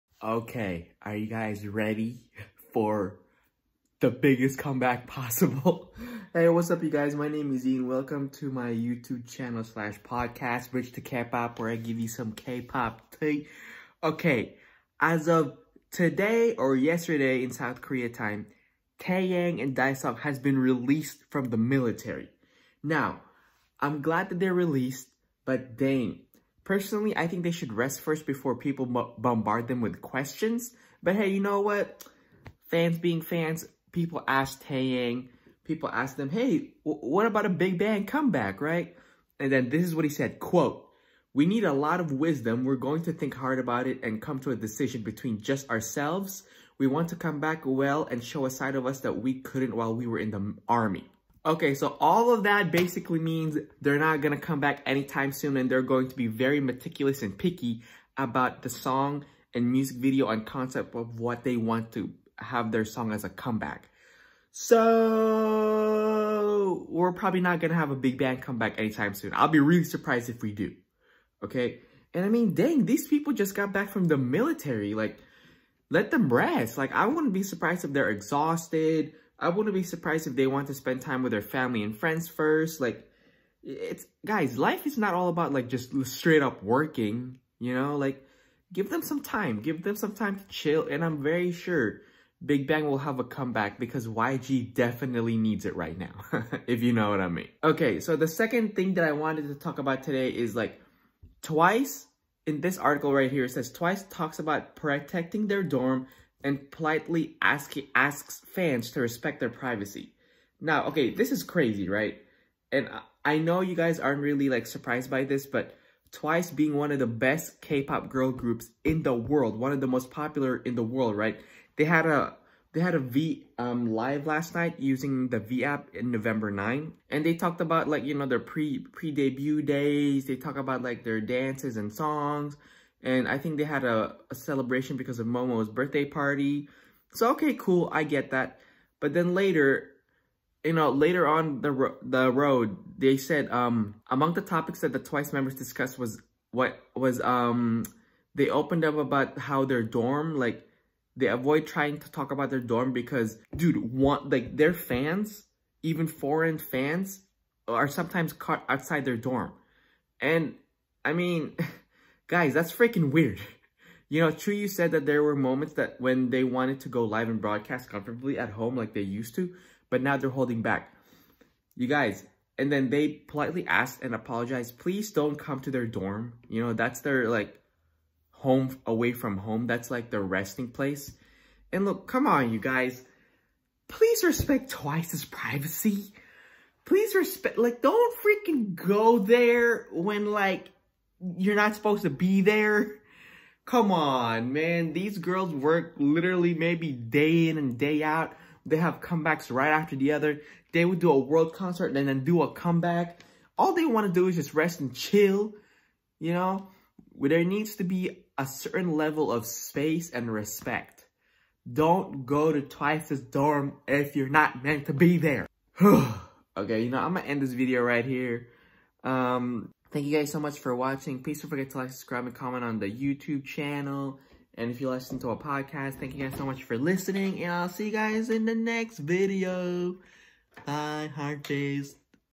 Taeyang and Daesung is back and Twice is very scared at their own dorm
EP. 48- Bridge to Kpop Podcast
Taeyang talks about possible big bang comeback and Twice is very scared in their own dorm